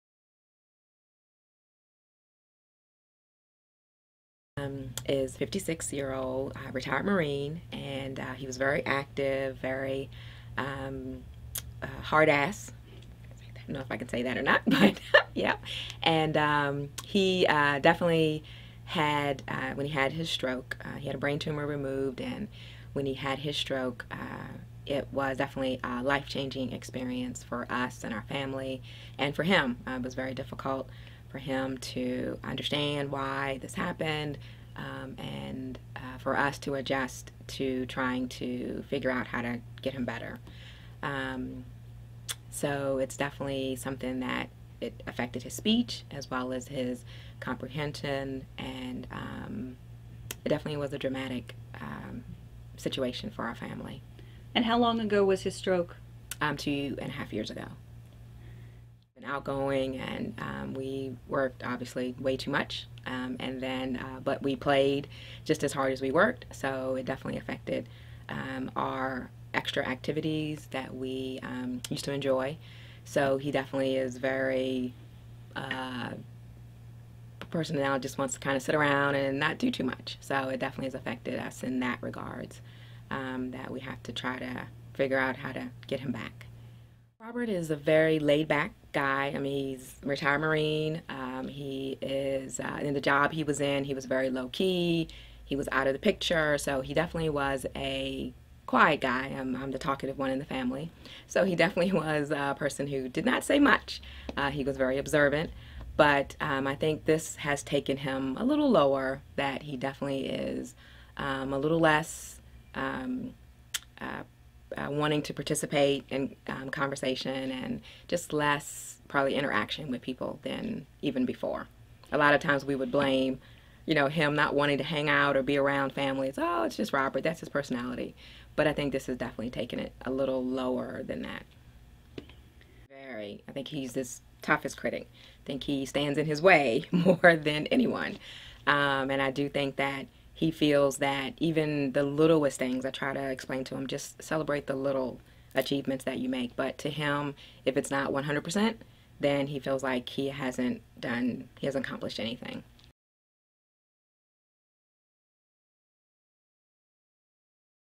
Robert: A Caregiver's Story of Severe Broca's Aphasia with Apraxia
This is the story of a 56-year-old retired Marine with aphasia and apraxia. Robert's communication was restricted to "to" and "ah-ah-ah" after his stroke. He didn't want to socialize anymore and is his toughest critic.